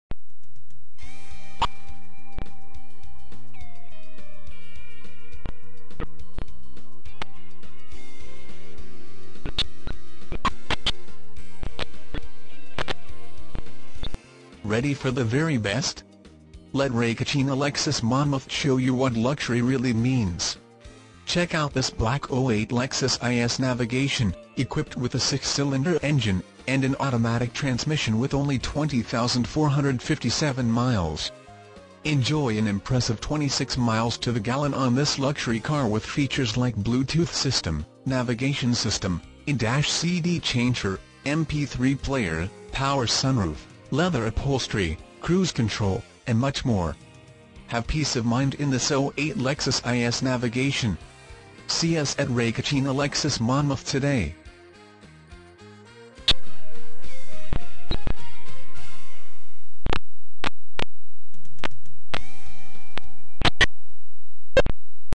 2008 Lexus IS Navigation Ray Catena Lexus Monmouth
2008 Lexus IS Navigation- Ready for the very best? Let Ray Catena Lexus Monmouth show you what luxury really means. You will love this Black 2008 Lexus IS Navigation, equipped with a 6 Cyl. engine and an automatic transmission with only 20,457 miles. Enjoy an impressive 26 miles to the gallon on this luxury car with features like Bluetooth System, Navigation System, In-Dash CD Changer, MP3 Player, Power Sunroof, Leather Upholstery, Cruise Control, and much more. Have peace of mind in this 2008 Lexus IS Navigation. See us at Ray Catena Lexus Monmouth today!
Miles-  20,457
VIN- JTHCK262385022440

Ray Catena Lexus Monmouth
2135 Highway 35
Oakhurst, New Jersey 07755